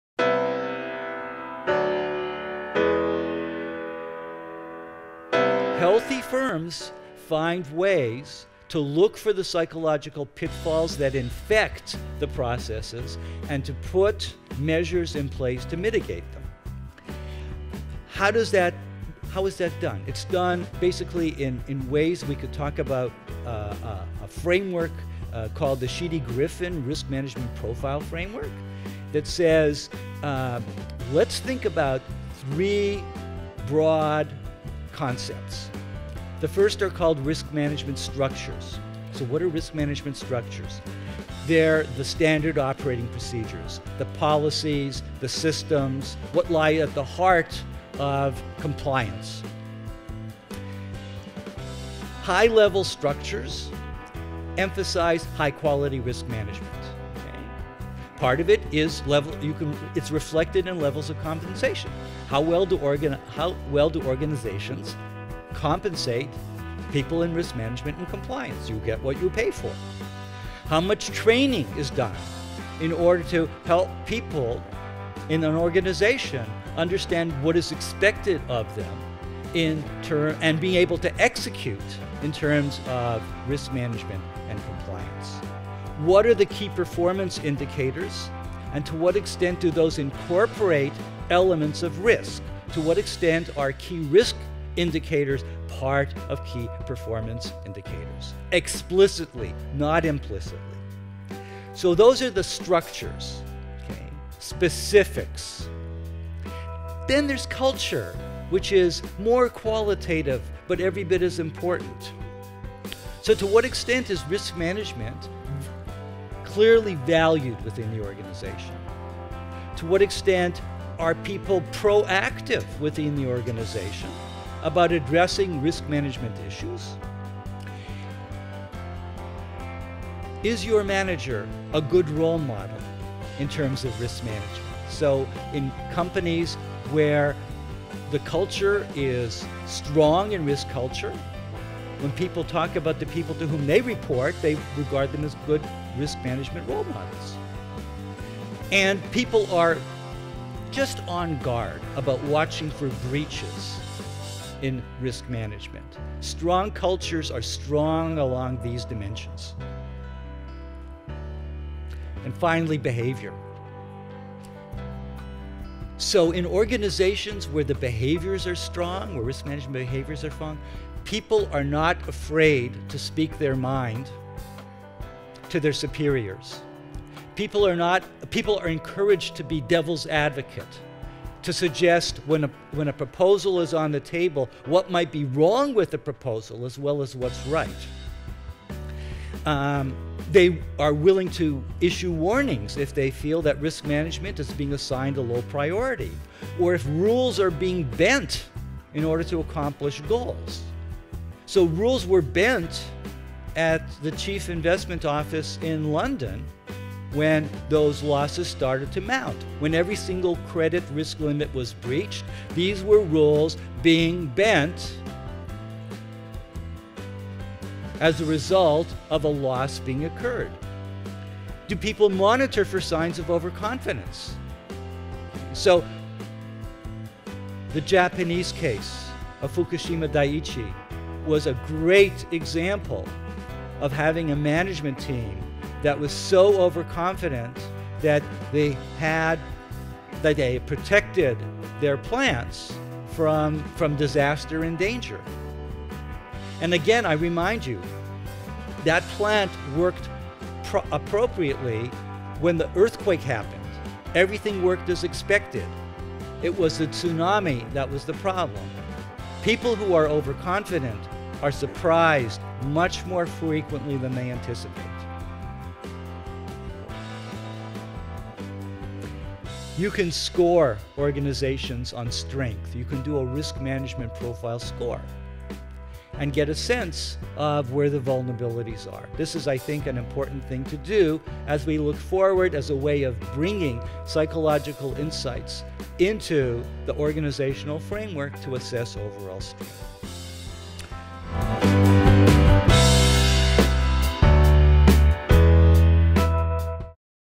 Hersh Sheferin en la conferencia Behavioral Risk & Compliance "El individuo y los sistemas"
El Profesor Hersh Shefrin en la Conferencia Behavioral Risk & Compliance "El individuo y los sistemas", habla sobre qué zonas del cerebro se ven involucradas a la hora de tomar decisiones. También nos cuenta como se ven a sí mismos dentro de las empresas los CROs y los COs.